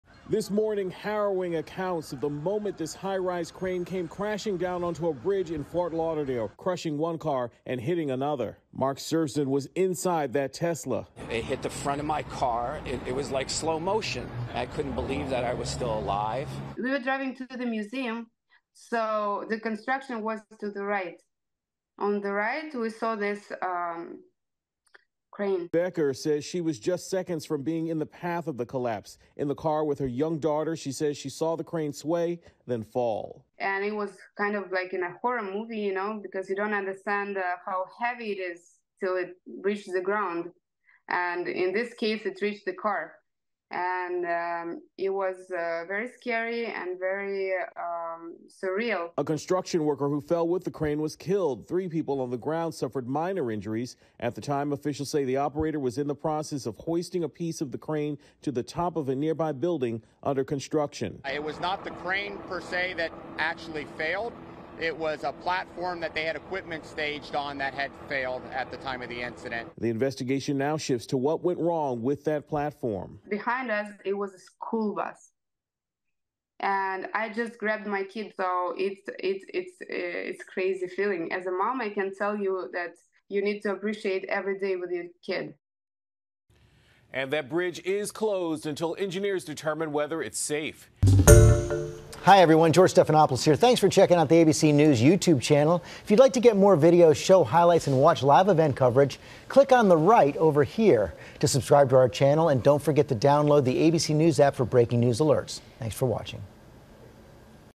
Harrowing accounts of deadly Fort Lauderdale crane collapse
A worker was killed after part of a crane came crashing down onto a bridge in Fort Lauderdale. ABC News’ Derricke Dennis spoke to a driver nearly crushed.